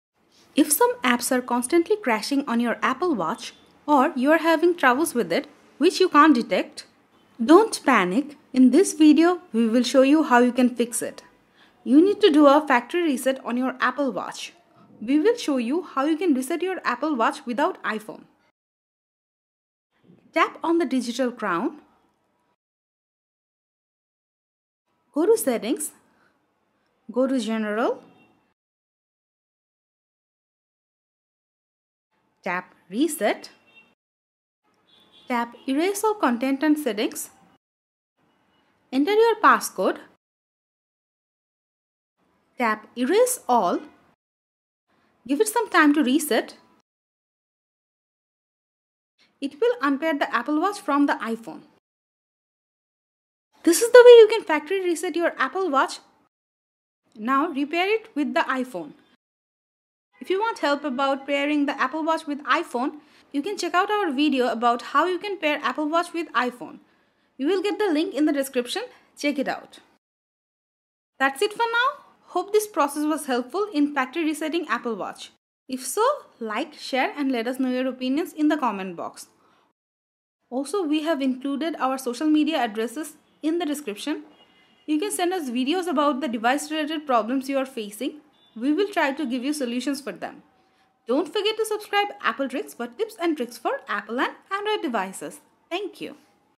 Factory Reset Apple Watch without iPhone
Want to Know how to factory reset your Apple Watch (Series 1, 2, 3, 4, 5) without iPhone and fix the issue when the Apple Watch stuck or frozen and won't move? In this video, you'll learn the easy steps of how you can reset your Apple Watch without iPhone.

Though if you do a FactoryResetAppleWatch, it will remove all contents and settings like music, data, messages, etc. everything else, still you have to do it to get back your Apple Watch in the previous condition.